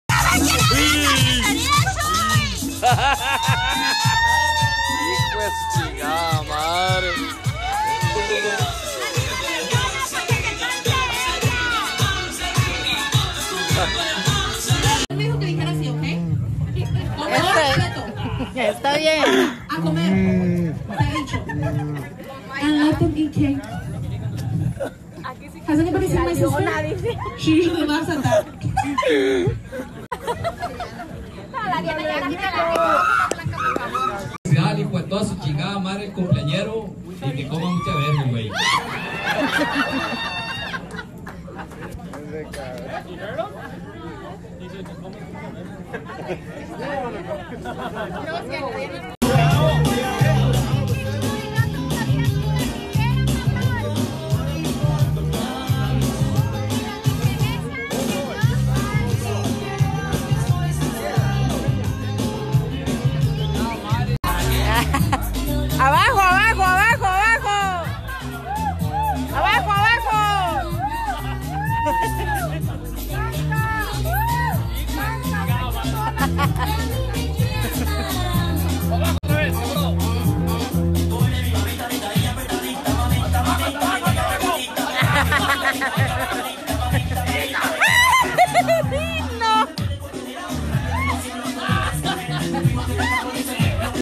ToroMax en la party y el Chuy hace el ridiculo (termina mal)
video del ToroMax extraído del Instagram de Chuy Patabionica